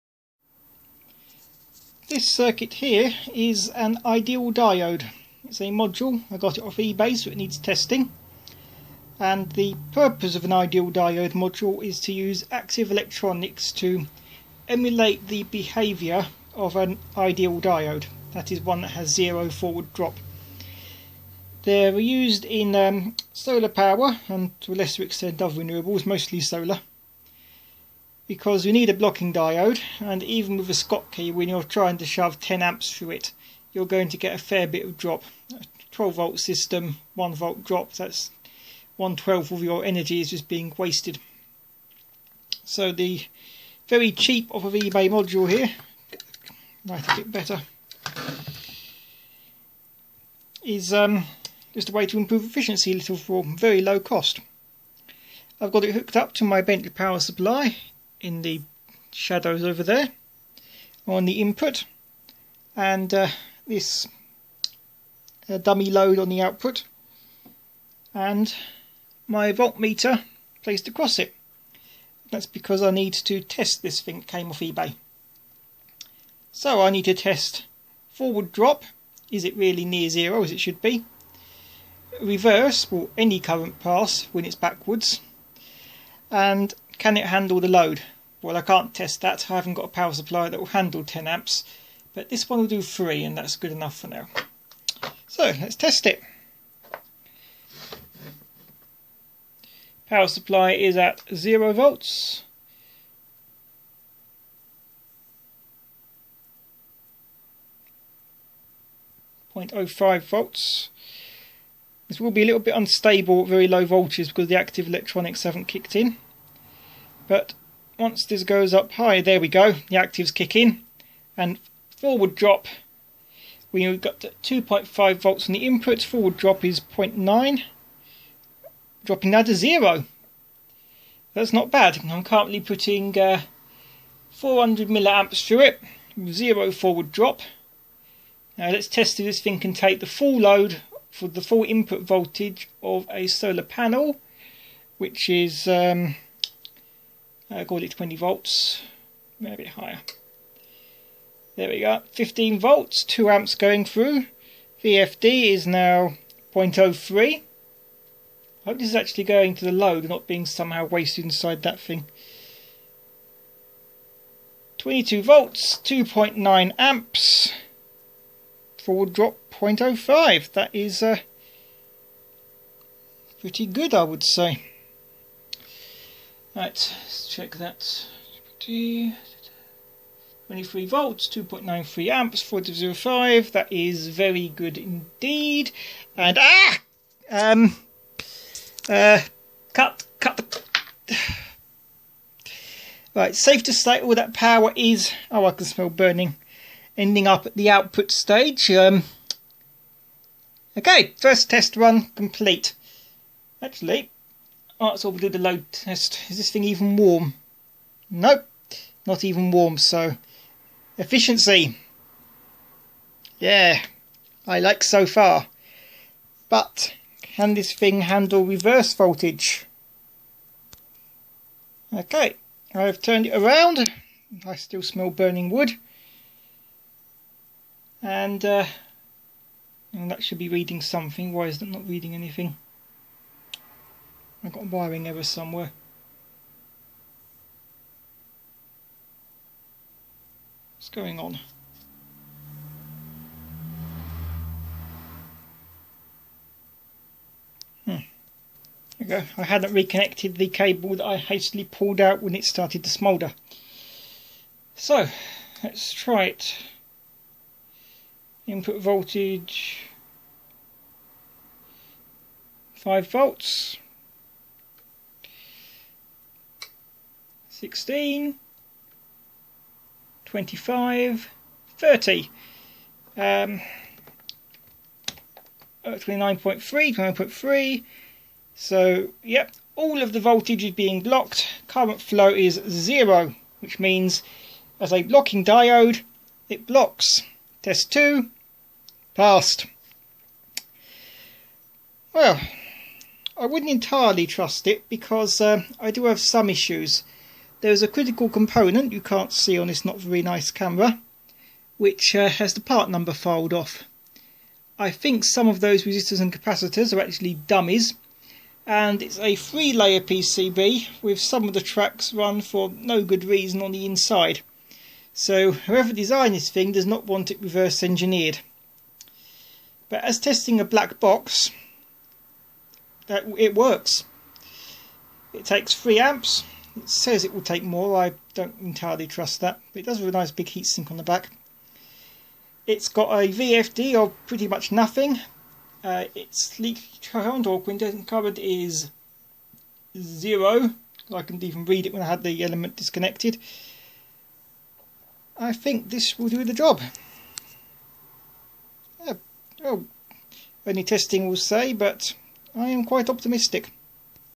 Testing an eBay ideal diode module.
Crude tests, but it does seem to work as advertised.